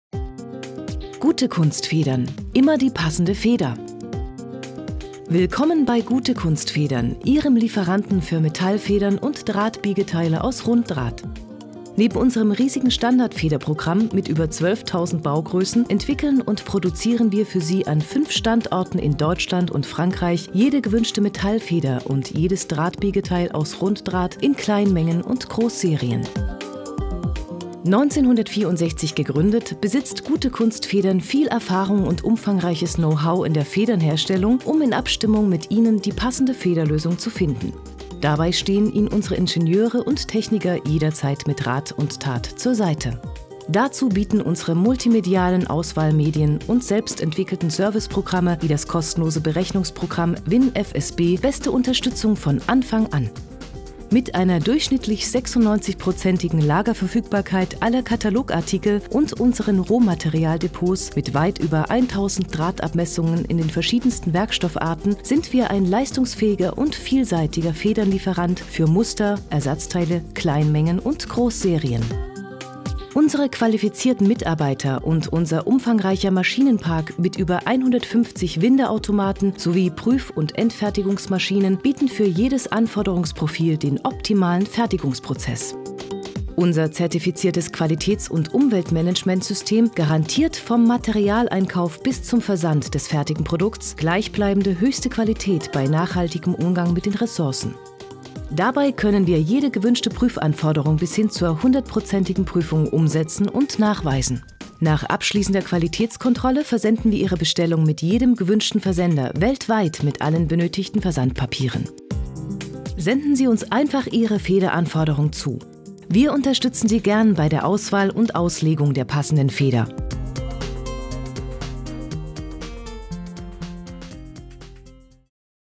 Gutekunst Federn Kurzvorstellung
Gutekunst Federn ist auf die Entwicklung und Fertigung von Druckfedern, Zugfedern,  Schenkelfedern sowie Drahtbiegeteilen aus jedem gewünschten Federstahldraht spezialisiert.

Neben dem umfangreichen Lagerprogramm mit 12 603 Federbaugrössen, fertigt Gutekunst Federn jede gewünschte individuelle Metallfeder bis 12 mm Drahtstärke in Kleinmengen und Großserien. Mit 320 Mitarbeitern beliefert das 1964 gegründete Familienunternehmen weltweit rund 100 000 Kunden aus den unterschiedlichsten Branchen. Zertifiziert nach ISO 9001 und 14001 zählt Gutekunst Federn heute mit fünf Niederlassungen in Deutschland und Frankreich zu den größten Federnherstellern in Europa.

bietet Gutekunst Federn alles für den Federnbedarf. Mit dem umfangreichen Federnkatalog, der individuellen Federnberechnung WinFSB, dem CAD-Daten-Generator und zahlreichen Informationen rundum Metallfedern bleibt keine Frage offen. ein Informations- und Frageportal rumdum „Metallfedern".